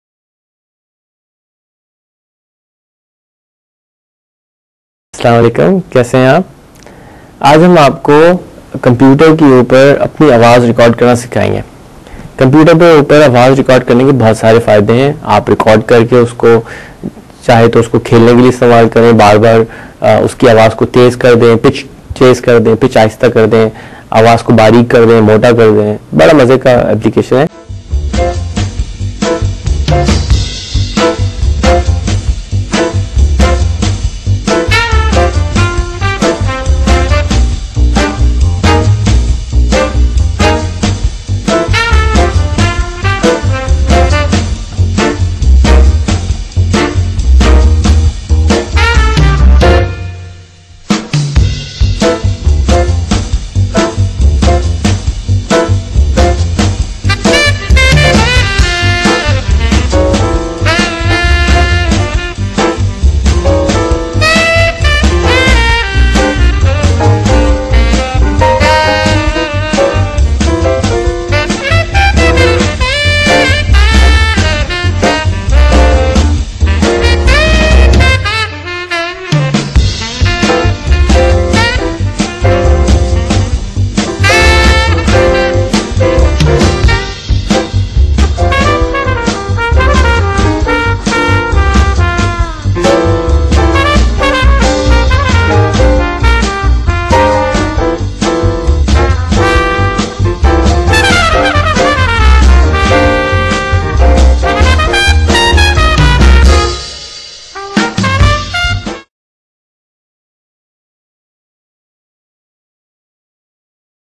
How To Record Sound
In this video you can learn how to record sound using your Pc

vid3010008